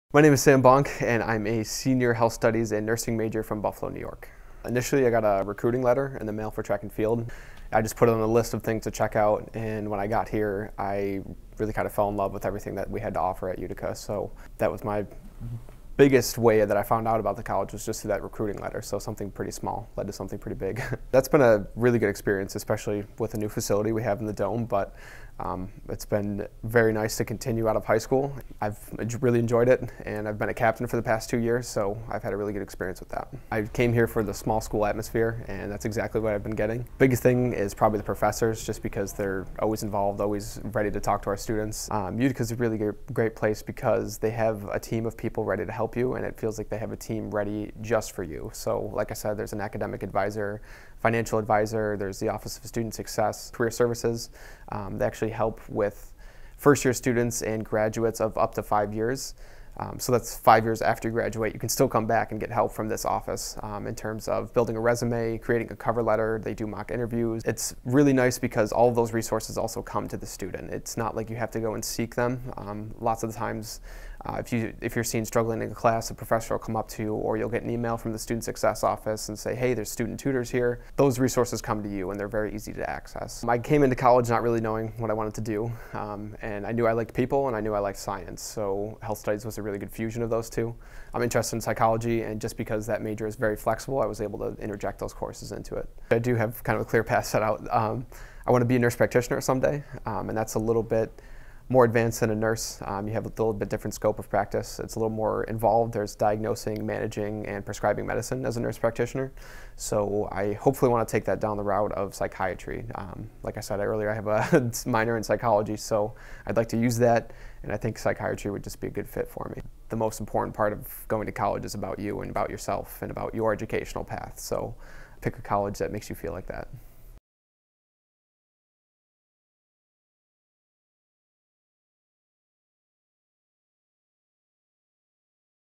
Profile: Samuel Bonk
A brief visit with Health Studies major Samuel Bonk about how he discovered Utica College, what he likes about his program, and where he plans to take his career after graduation.